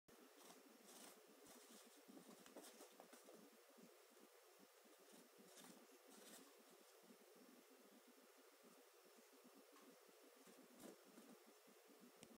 whiffles loves teh dust
my chinchilla mr. whiffles doing his thang. yeah i think he does crack.